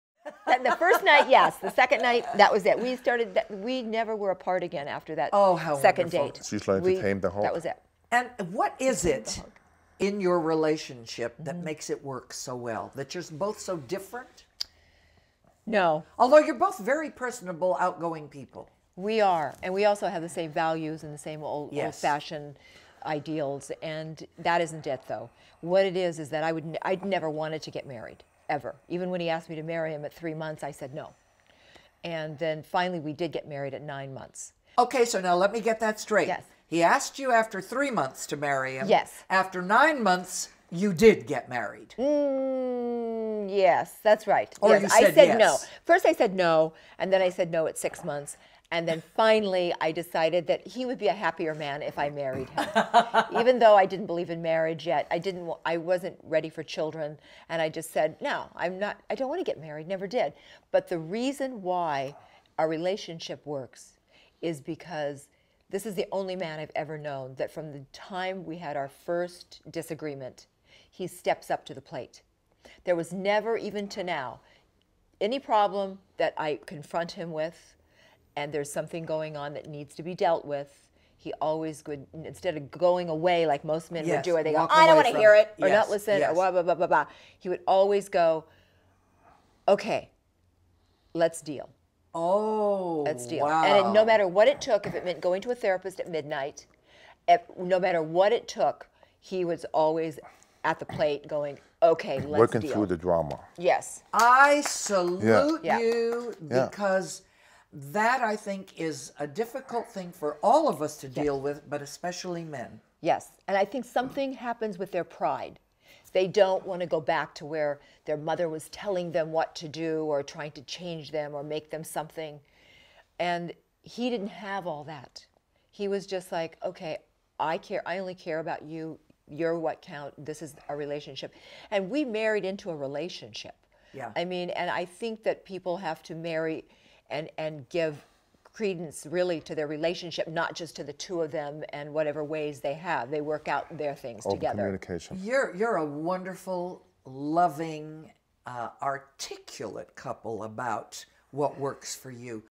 "The Incredible Hulk" Lou Ferrigno with wife Carla on what makes their marriage special
"The Incredible Hulk" Lou Ferrigno and his wife Carla explain to Ruta Lee for Lifestyle Magazine's Mad About Marriage about what makes their marriage special and their dedication to communicating with each other that keeps their long-term marriage strong. Great example of how to communicate well in your marriage.  Get more information about how to improve your marriage